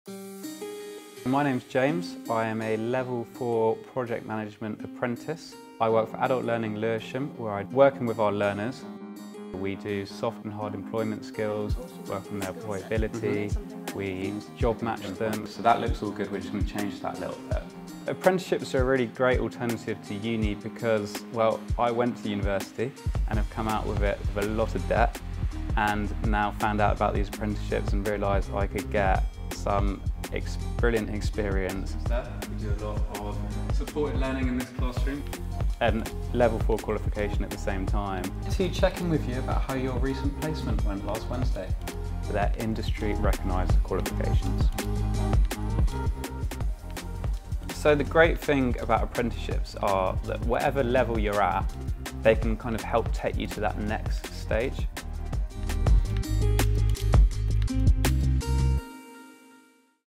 National Apprenticeship Week 2023: James
It's National Apprenticeship Week and we are celebrating the fantastic opportunities that apprenticeships offer. Meet James - a business apprentice in our Adult Learning Lewisham team.